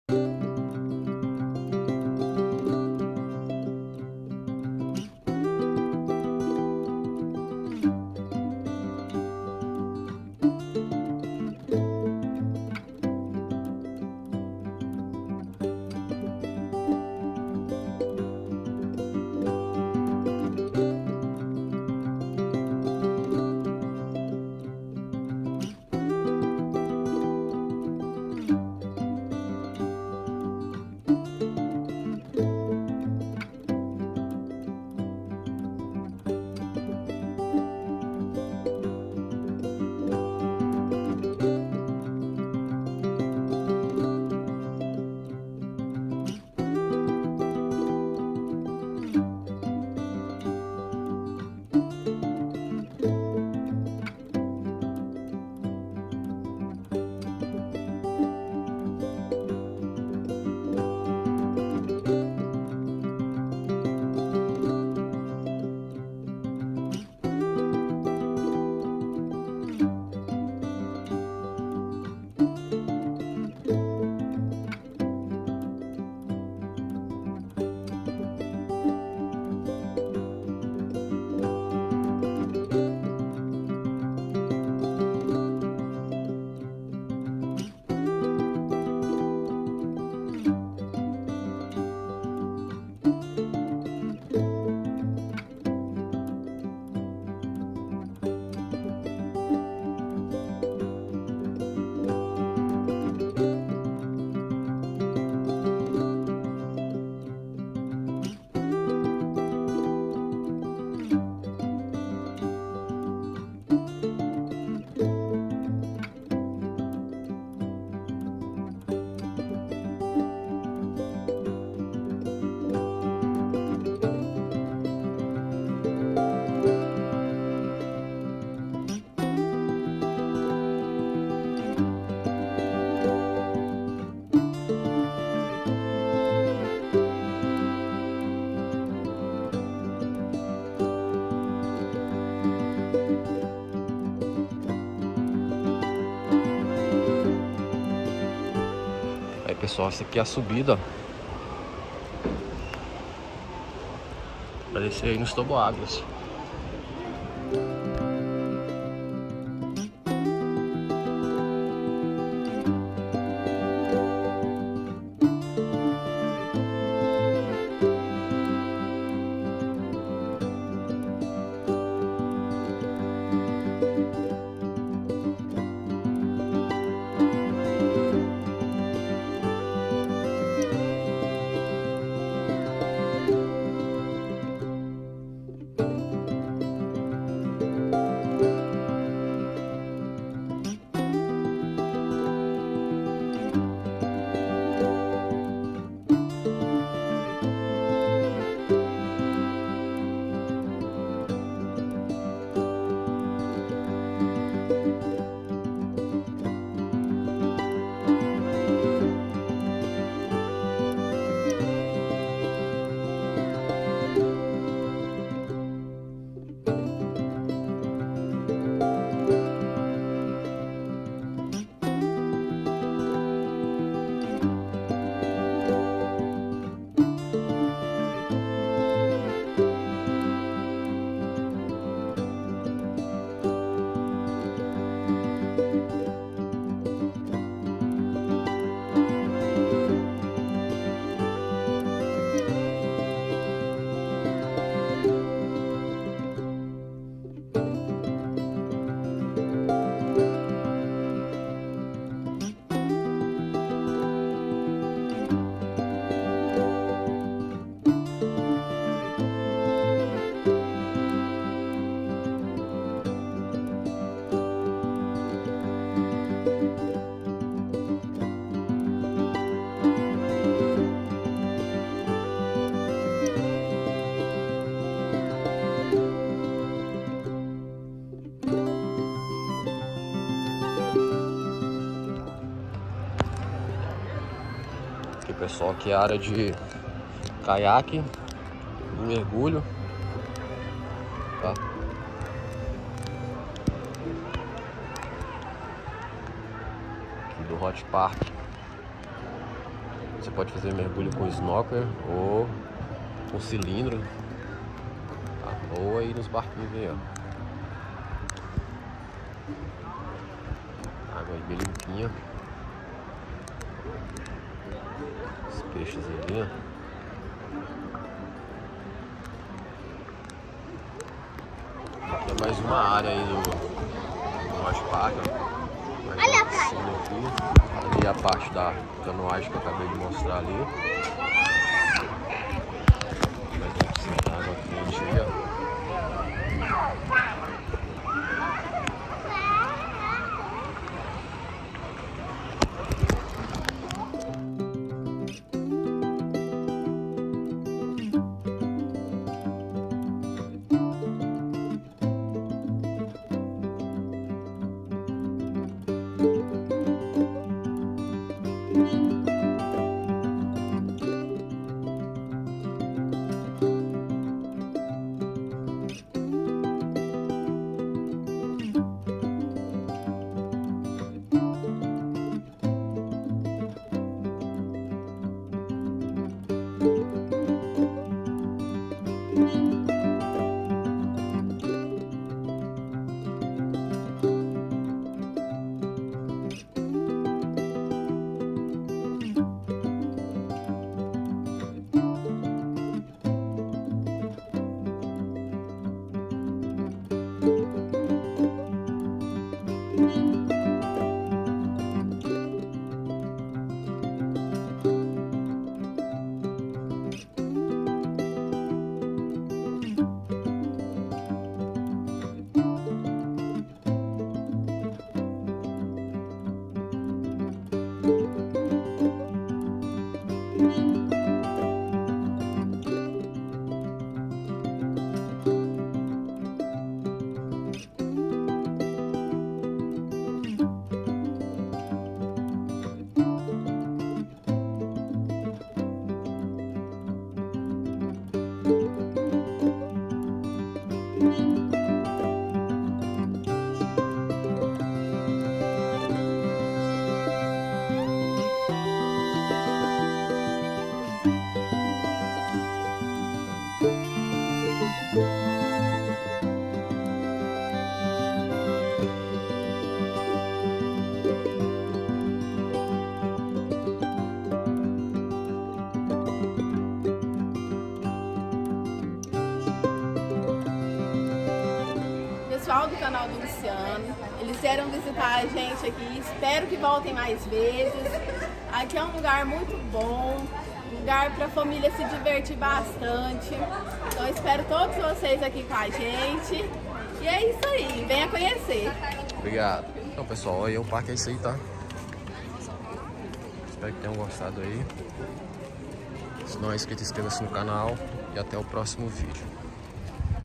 Hot Park Rio Quente - Caldas Novas - GO 2022
Neste vídeo mostrarei um resumo do Hot Park, que fica no Rio Quente, próximo à Caldas Novas - GO. Espero que ajude a sanar dúvidas de quem nunca foi ao lugar. Dúvidas, postem nos comentários.